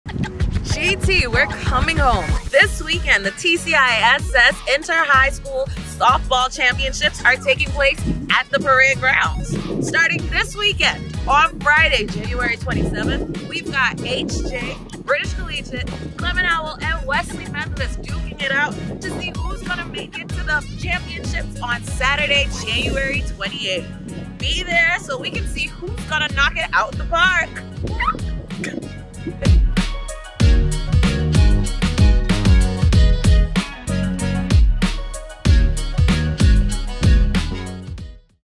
TCISS Inter-High School Softball Championships 2023 - Grand Turk
LETS GOOOOOO!! 🥎💥

Good vibes, good sports and teamwork all in the Nation's Captial for the TCISS Inter-High School Softball Championships this weekend.

Here is the schedule 📅
Fri Jan 27 | 𝐒𝐄𝐌𝐈-𝐅𝐈𝐍𝐀𝐋𝐒 | 🎟️: Adults $10, Students $5
Semi-Final 1 BWIC vs HJRHS | 6PM
Semi-Final 2 CHHS vs WM | 8PM
📍: Parade Grounds, Grand Turk

Sat Jan 28 | 𝐂𝐇𝐀𝐌𝐏𝐈𝐎𝐍𝐒𝐇𝐈𝐏 | 🎟️: Adults $10, Students $5
Third Place | 4:30PM
Championship Game | 6:30PM